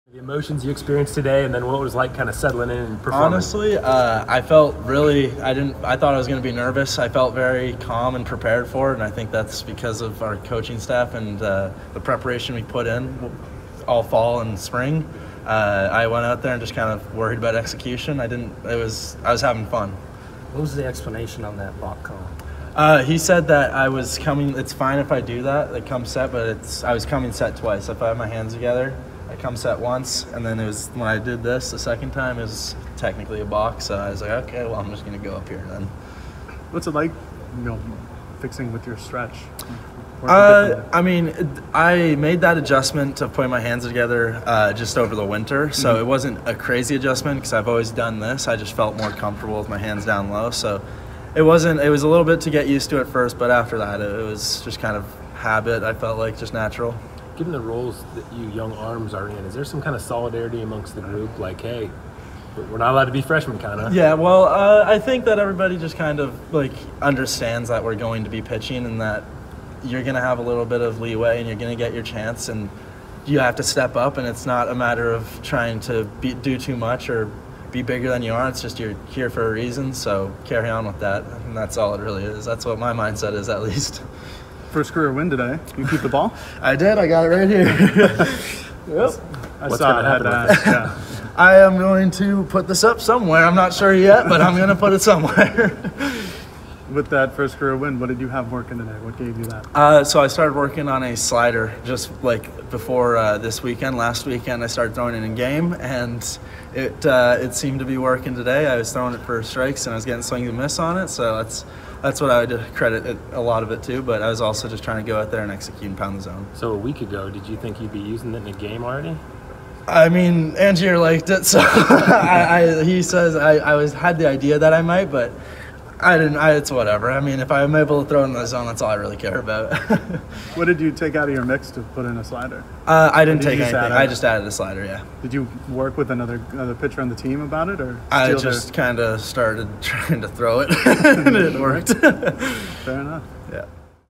Turner Spoljaric | Postgame vs. Xavier | Games 2 & 3
#32 Fr. RHP Turner Spoljaric meets with the media following Oregon's doubleheader sweep of Xavier on Saturday.